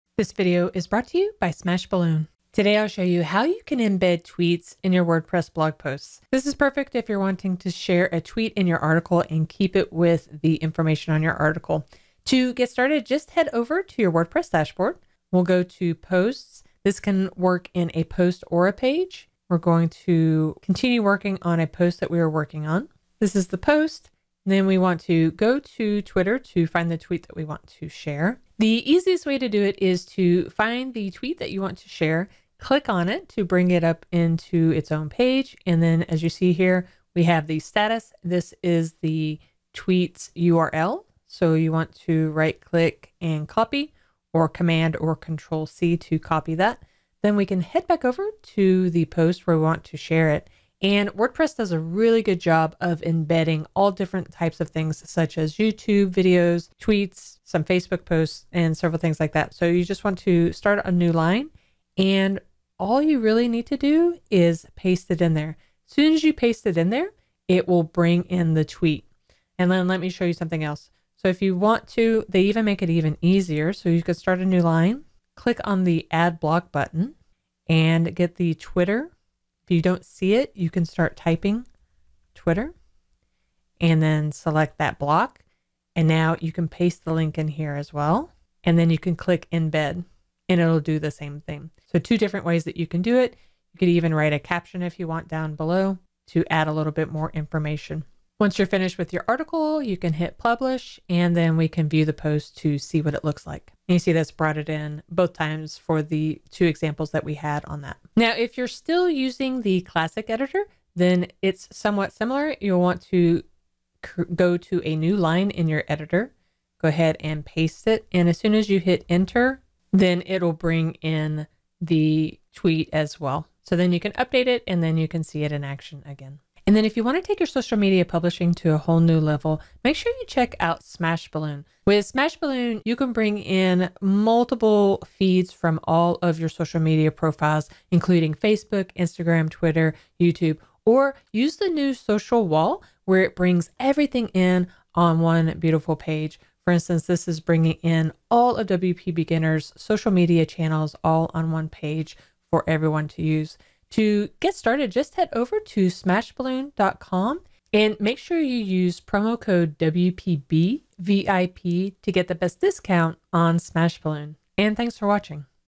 How to Embed Tweets in WordPress Blog Posts
Do you want to embed specific tweets on your WordPress posts? These embeds allow you to easily quote tweets inside your content to help make it more engaging. In this video, we will show you how to easily embed tweets in your WordPress posts and pages.

--Top Resources--

For our discount, use our code: WPBVIP

For this guide, we will first want to go to the tweet we are wanting to copy. There should either be a dropdown arrow in the top right of the tweet or you can click on the time of the tweet to go directly to that tweet's page.

Copy the link from the top of the page and go to your WordPress admin area. Dy default, if you add the link to your content on a separate line it should automatically create the embed.

For another method using the default Block editor, we will add the Twitter block, this should bring up a block where we can add the twitter URL and click the embed button to generate the embed.